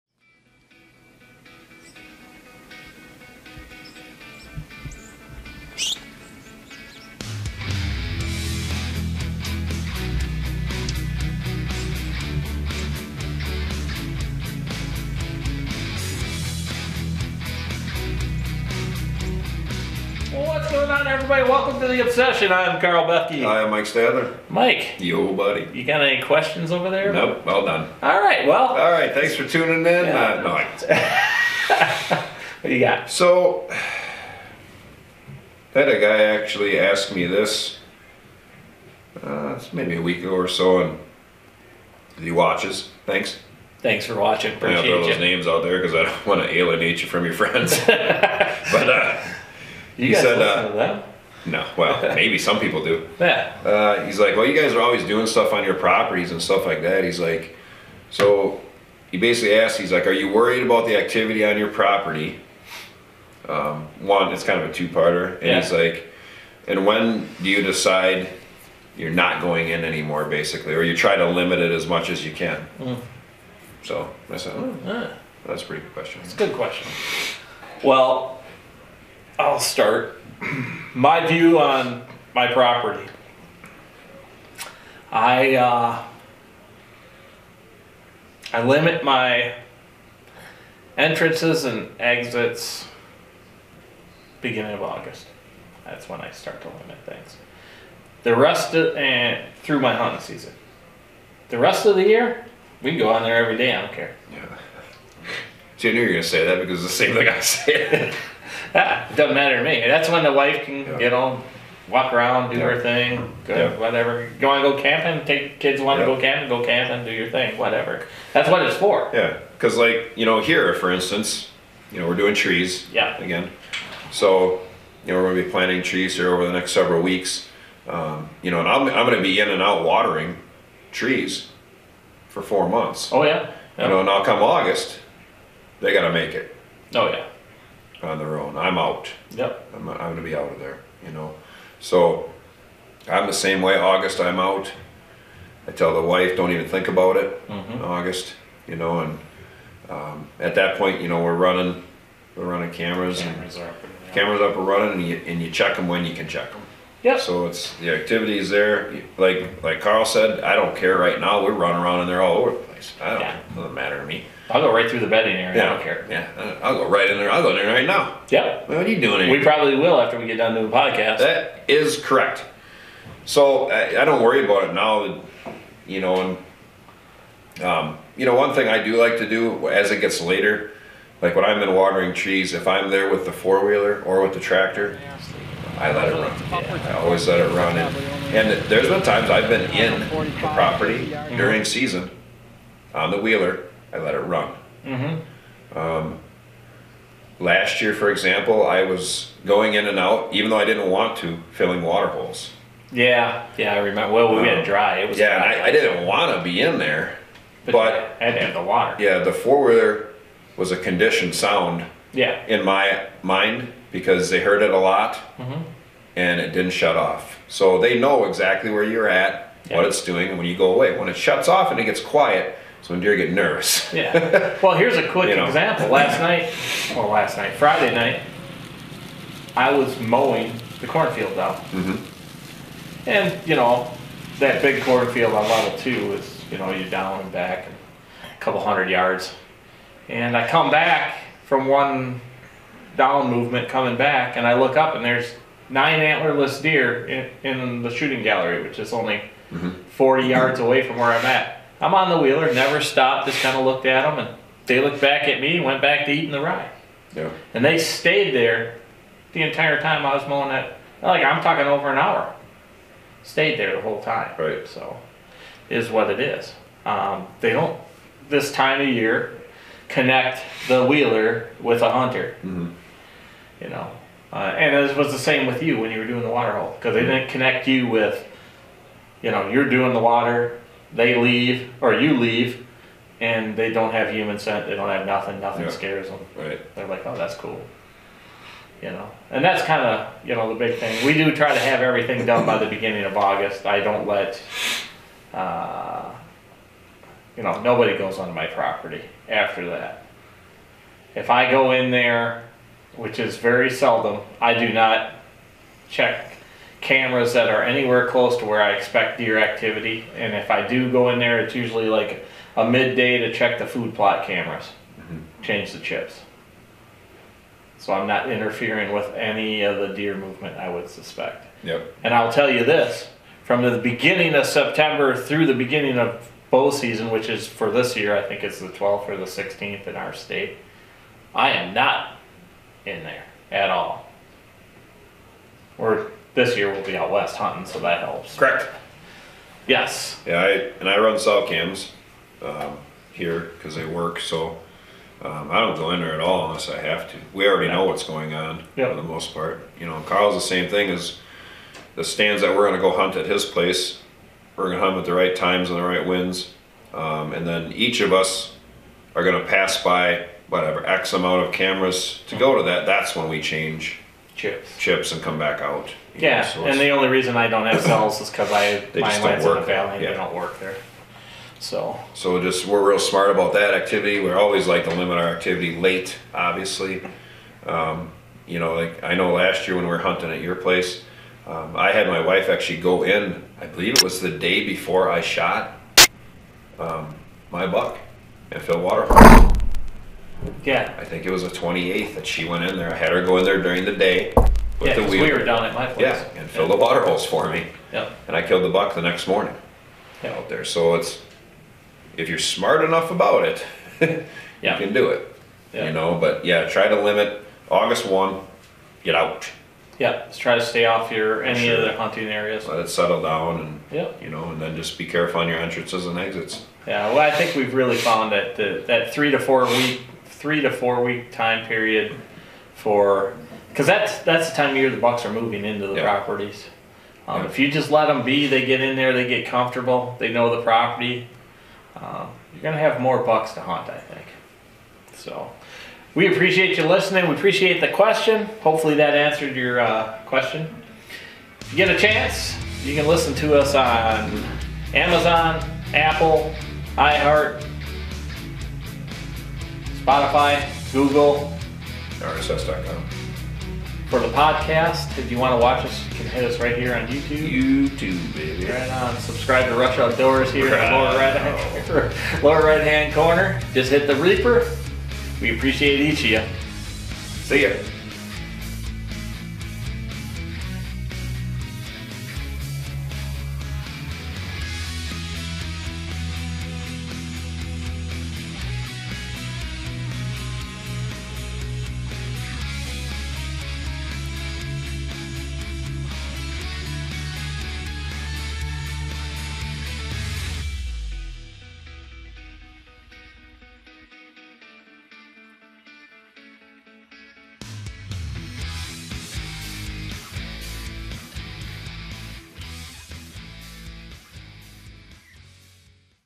When to limit your intrusion on your land for a good fall hunt s2e28
This week you asked Karl, and Mike when do they think you should start to limit your intrusion on your hunting properties? Well the guys have a set date each year when they believe you need to stop being intrusive on your land to have a great hunt in the fall. Karl , and layout exactly what they do to be less intrusive were they go, and were they do not go to have the best chance of taking a good buck during the upcoming hunting season. Leave a comment let us know when you stop being intrusive  on your hunting spots. we want to hear from you.